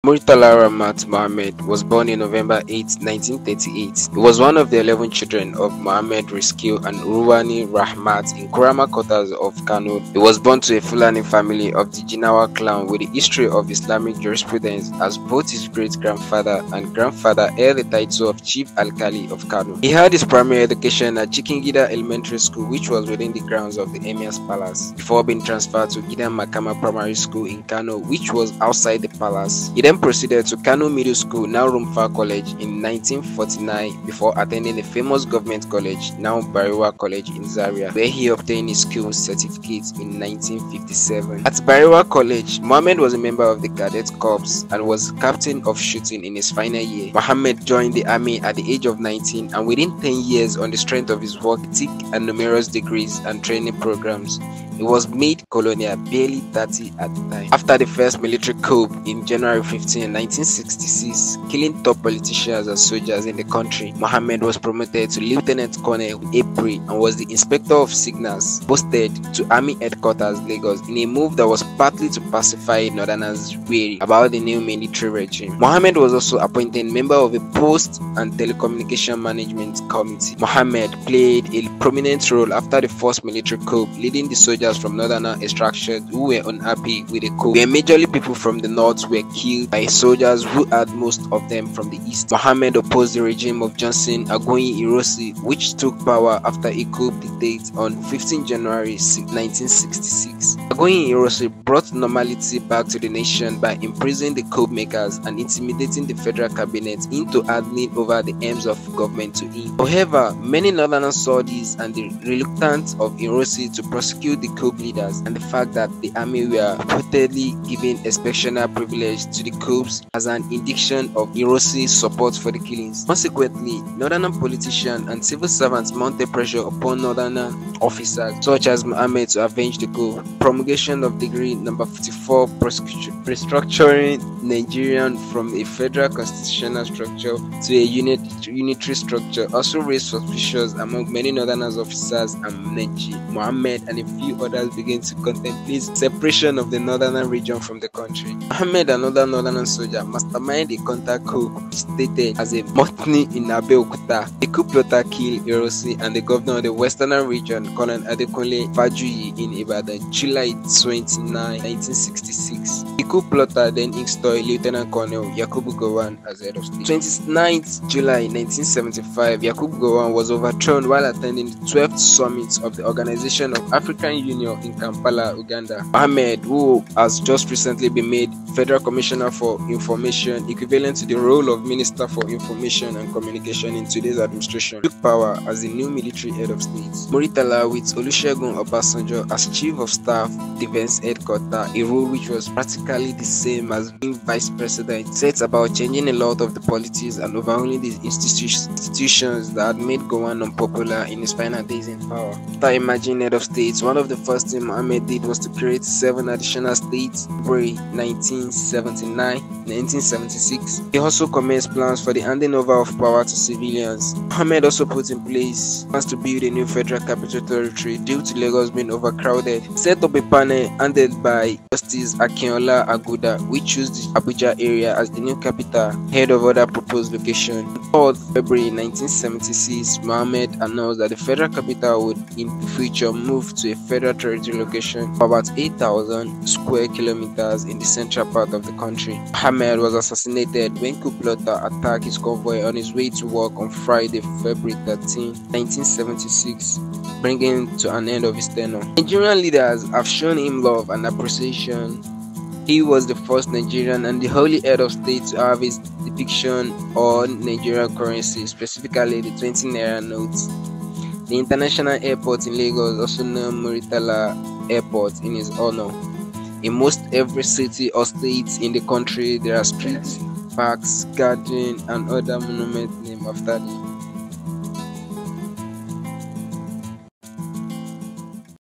Nigeria’s Finest General: General Murtala Muhammed Life and death
Muhammed played a prominent role after the first military coup, leading the soldiers from northern extraction who were unhappy with the coup, where majorly people from the north were killed by soldiers who had most of them from the east.

Muhammed opposed the regime of Johnson Aguiyi-Ironsi, which took power after a coup d’etat on 15 January 1966. Aguiyi-Ironsi, brought normality back to the nation by imprisoning the coup makers and intimidating the federal cabinet into handing over the helms of government to him. However, many northerners saw this and the reluctance of Ironsi to prosecute the coup leaders, and the fact that the army was purportedly giving exceptional privileges to the coupist as an indication of Ironsi’s support for the killings. Consequently, northern politicians and civil servants mounted pressure upon northern officers such as Muhammed to avenge the coup. The promulgation of Decree No. 34 restructuring Nigeria from a federal constitutional structure to a unitary structure also raised suspicions among many Northern officers and Mohammed and a few others began to contemplate separation of the Northern region from the country.

Muhammed and other northern soldiers masterminded a counter coup which started as a mutiny in Abeokuta. The coup plotter killed Ironsi and the Governor of the Western Region, Col. Adekunle Fajuyi in Ibadan July 29, 1966. The coup plotter then installed Lt. Col.